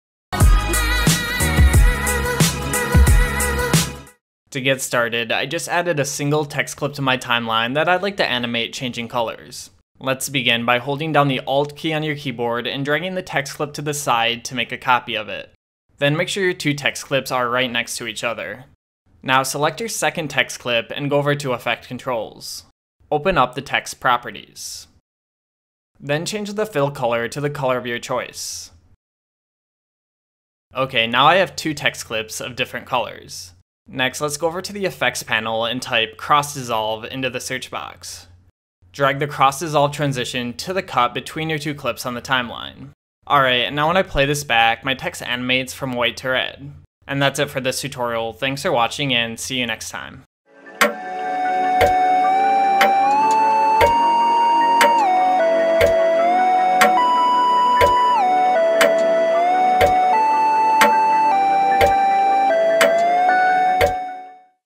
How to Animate Text Color Change | Premiere Pro Tutorial
A tutorial on how to make animated text that changes colors in Premiere Pro.  This lets you animate your text with two colors.